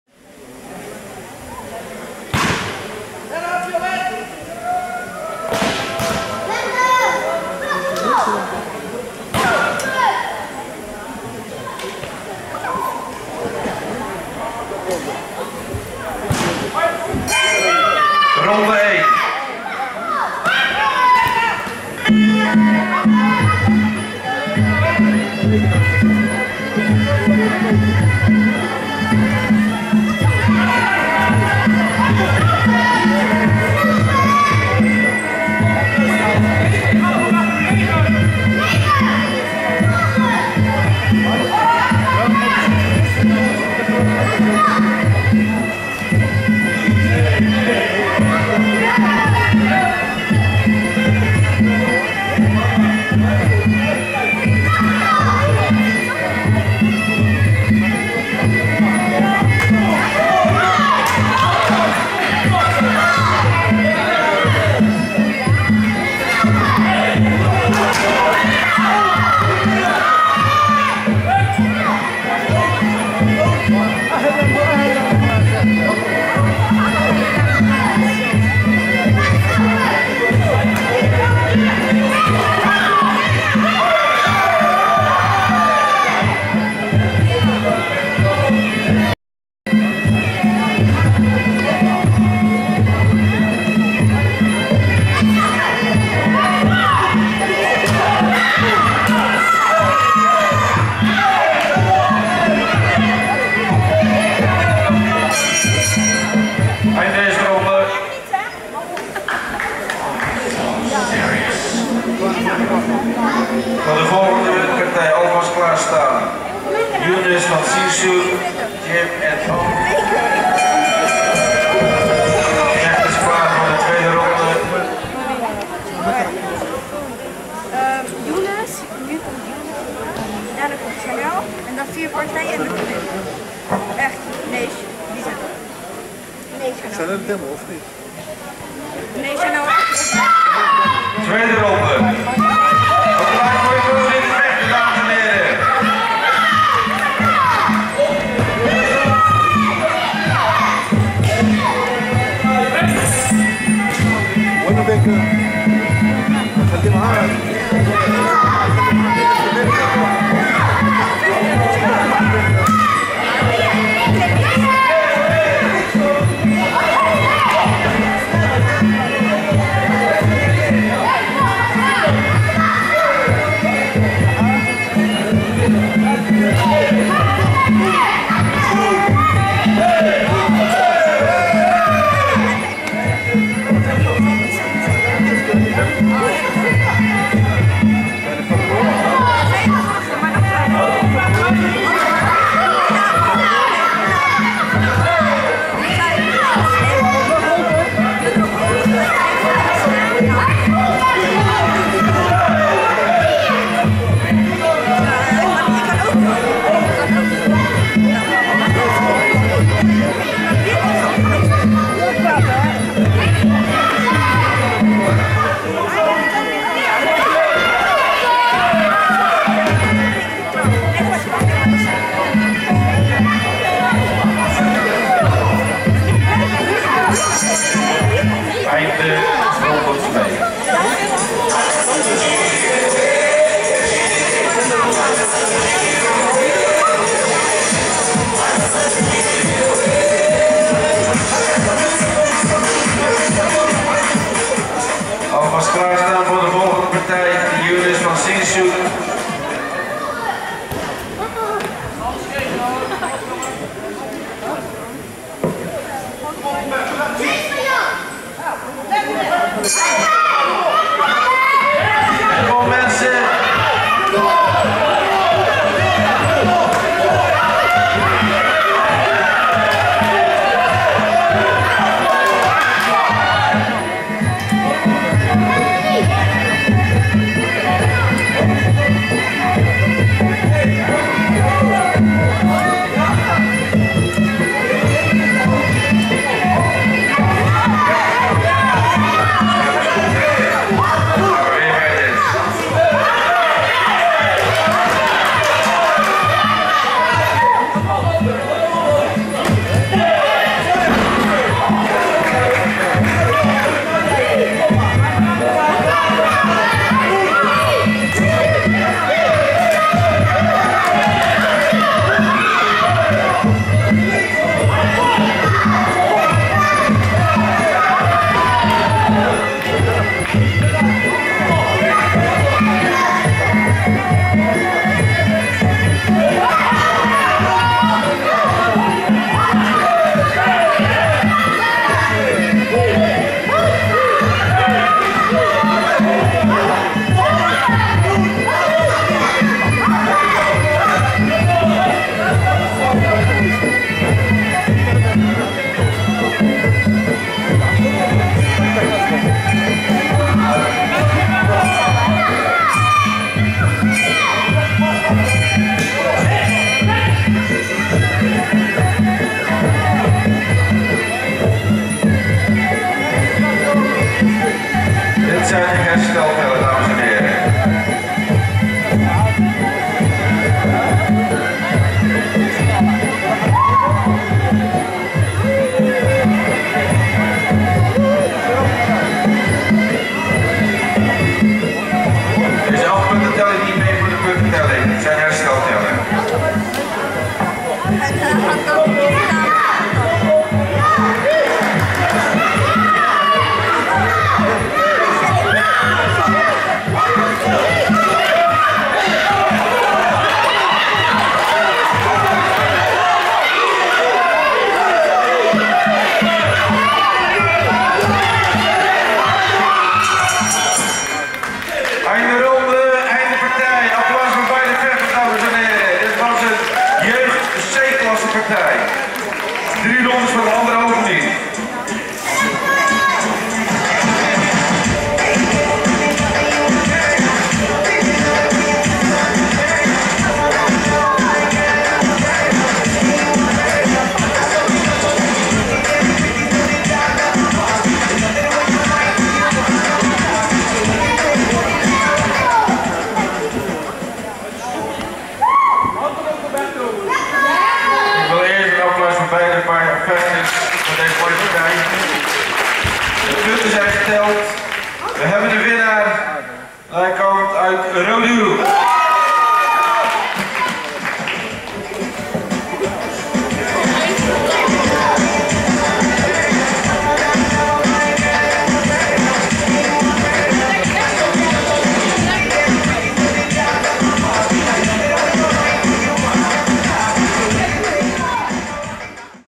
Roberto Zaandam vs Chatmongkong
Junior fight Muay Thai Roberto Zaandam (Fidan Gym) vs Chatmongkong (Hemmers Gym) @ Muay Thai event Diamond Gym, Diamond Gym Dordrecht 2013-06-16.